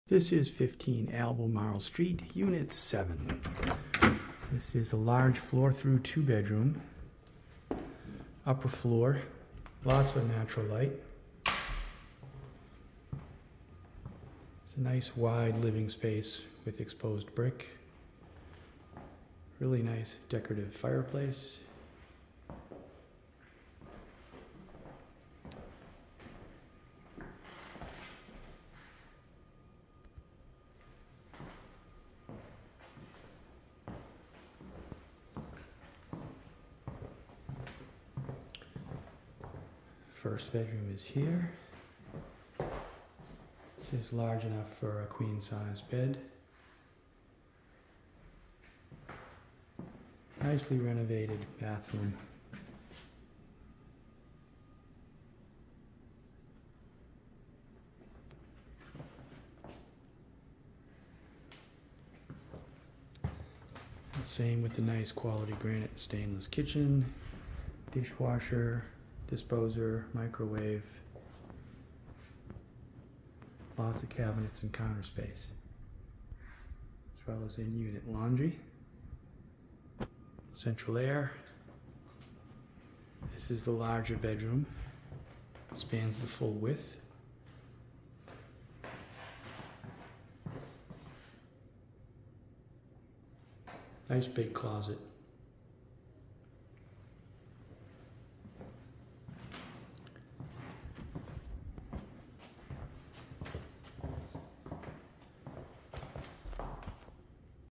15 Albemarle St #7 Walk thru Vacant
Here's a beautifully renovated two bed apartment with a great address! You're a two minute walk to the Orange line or the green line, the Prudential Center and lots of great South End restaurants and cafe's. This apartment features central AC, gorgeous hardwood floors and a well equipped, granite and stainless kitchen with gas cooking, dishwasher, disposal, microwave and laundry in the unit. There's a spacious, sunny living room with great city views and a decorative fireplace and two spacious bedrooms, though the second room is smaller. Heat and hot water are included in the rent.